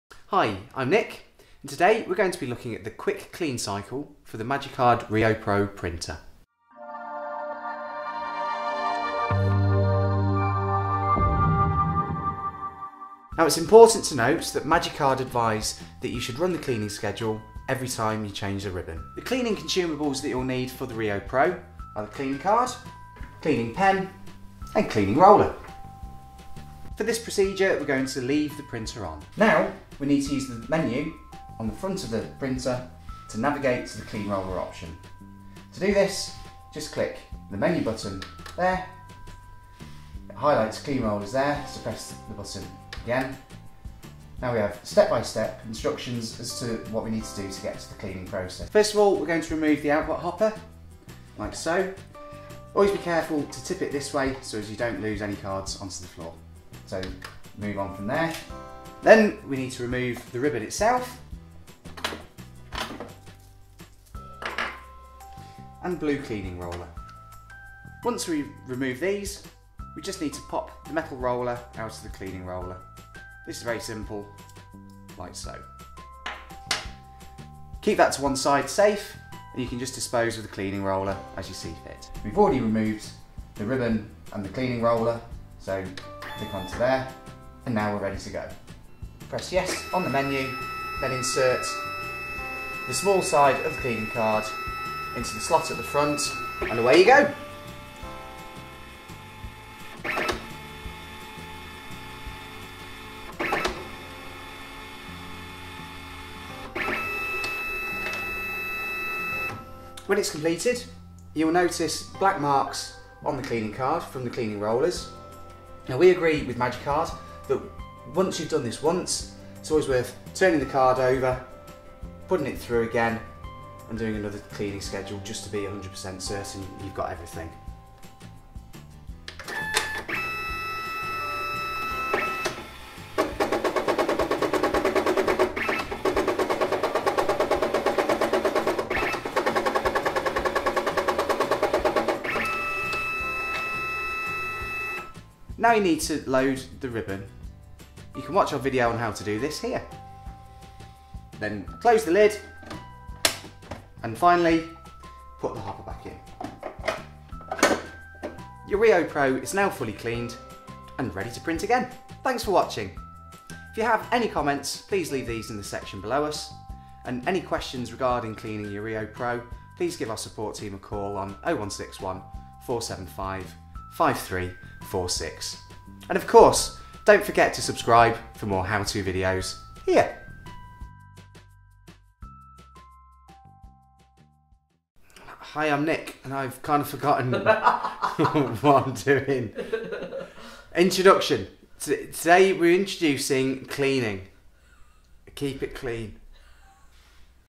How to do a Quick Clean on the Magicard Rio Pro ID Card Printer
In this short video, Nick, our ID card printer expert, will show you how to perform a quick clean on the Magicard Rio Pro plastic card printer.

He'll show you how to;

- Prepare the Magicard Rio Pro
- Perform a clean using the printer
- Do the quick clean
- And start printing again